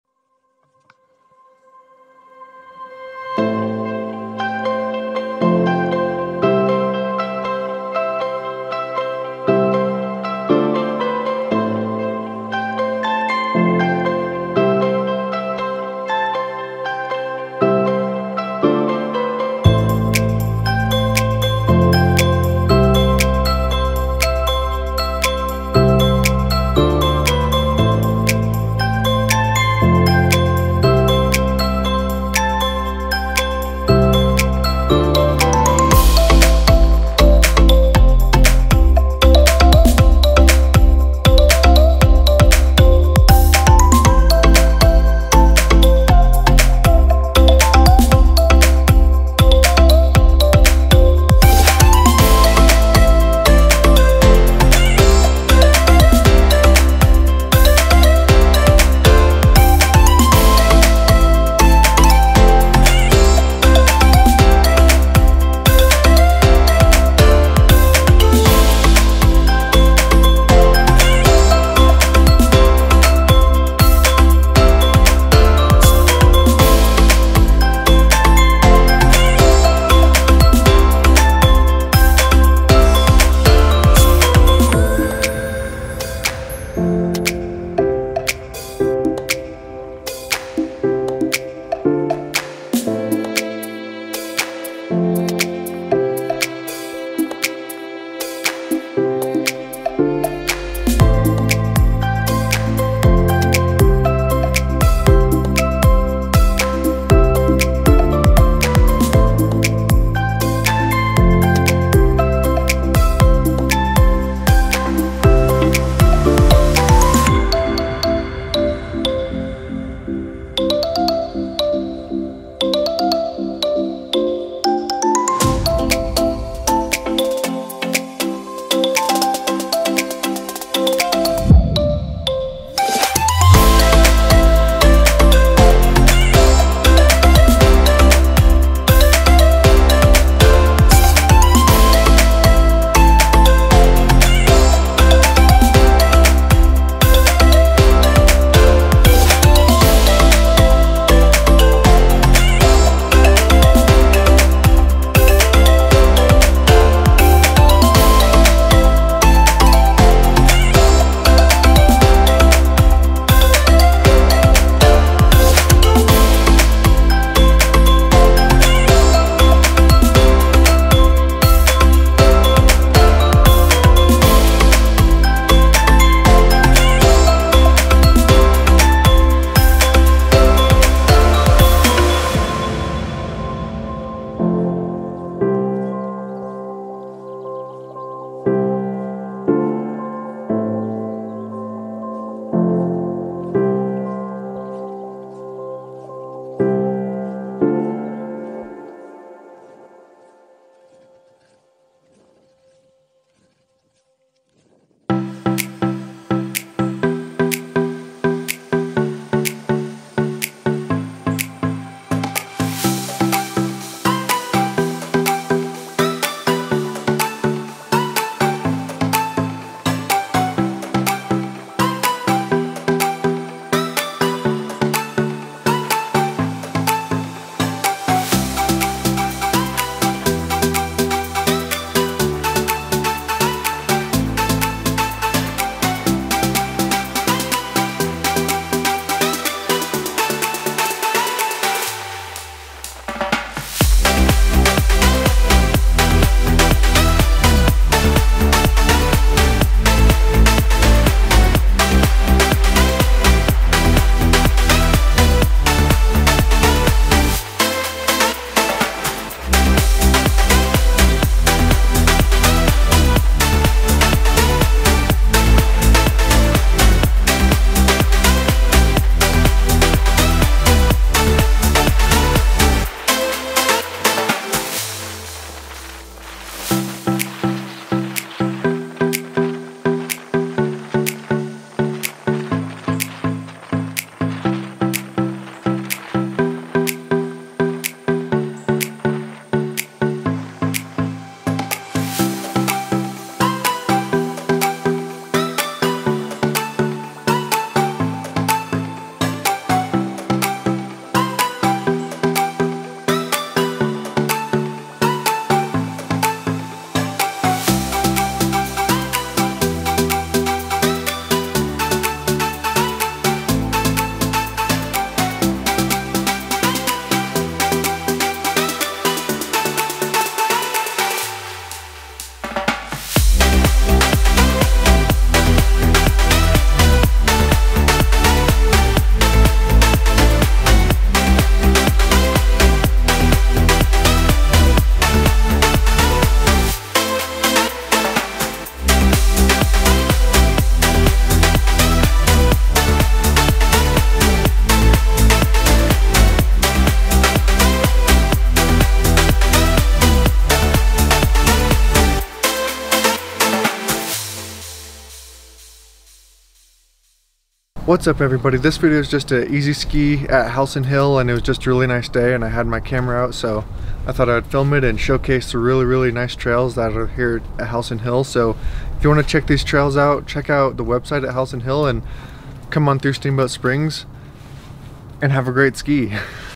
Cross Country Skiing in Steamboat Springs CO!!
Cross Country Skiing in Steamboat Springs CO at Howelsen Hill on a gorgeous day on excellent groomed trails!! Just a show case of some of the amazing Cross Country Skiing we have in Steamboat Springs CO! I hope to see you out here this winter!

Let me know if you have any questions about the trails, skis, gear, conditions, etc. in the comments below. Also any feed back on the video would be great!

Sorry for the wind noise, its a first edition 360 camera so I tried to hide it with loud songs.

Thanks!

I hope you get some great skiing!!